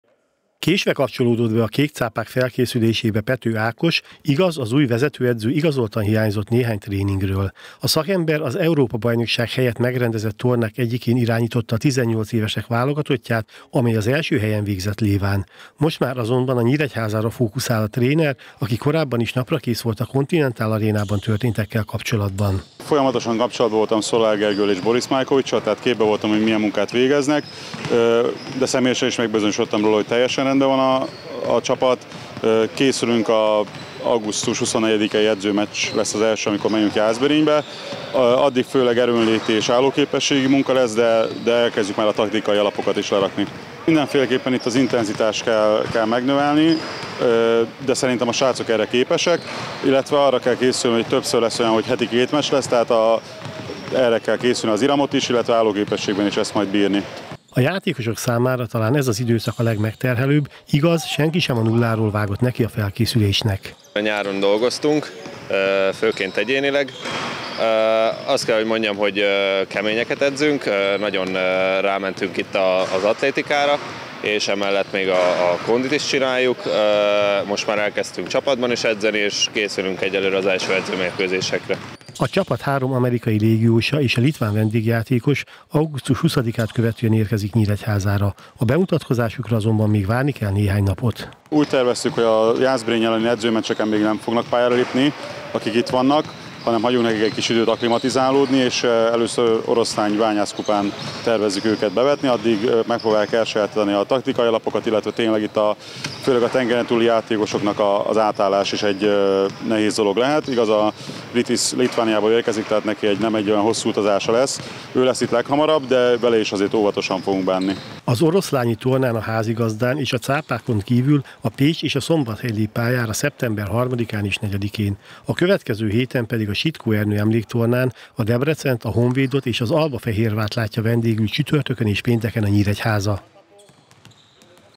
Pethő Ákossal készülnek
A héten már Pethő Ákos irányításával készül a Hübner-Nyíregyháza Blue Sharks.
A szakember a szlovákiai Léván az U18-as fiú válogatottal a FIBA korosztályos versenyén vett részt, ahol tornagyőztesek lettek. A Kék Cápák jövő kedden Jászberényben játsszák az idény első felkészülési meccsét.